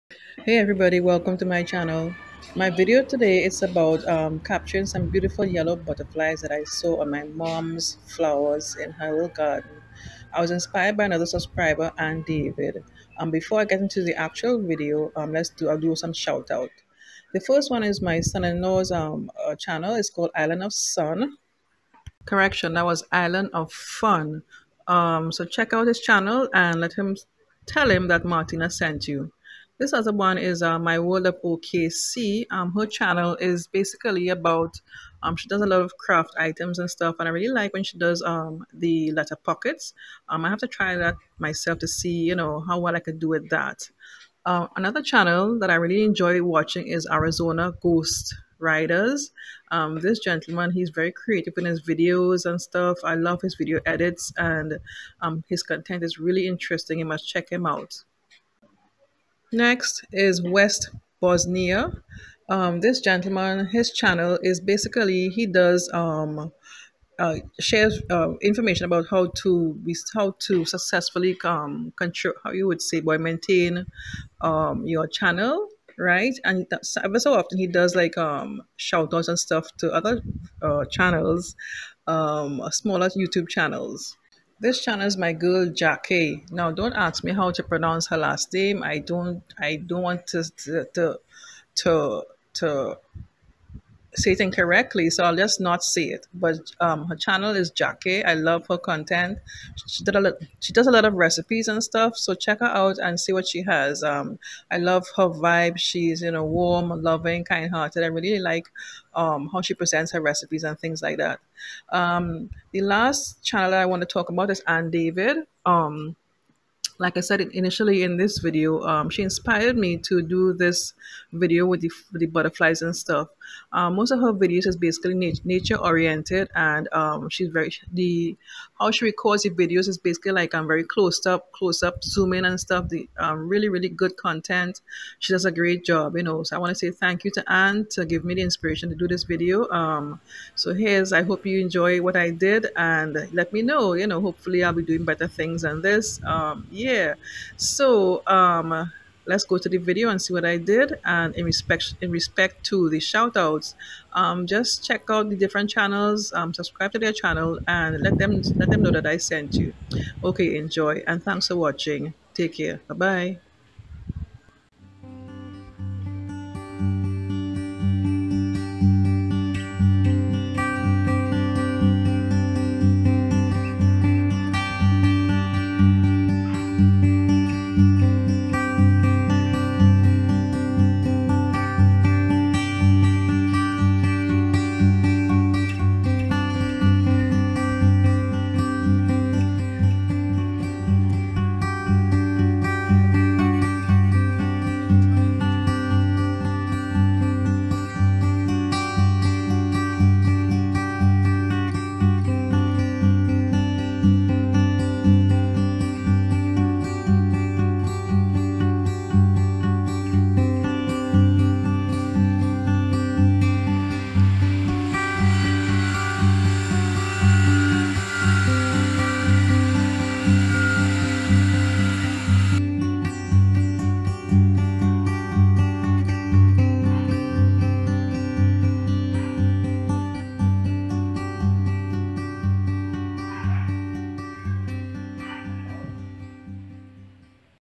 Beautiful Yellow Butterflies And Shout Outs💕💕
I recorded some beautiful butterflies in my mother's flower garden.  I got the inspiration to do this video, from another YouTuber Ann David.  I am also taking the opportunity to do a shout out!!💕💕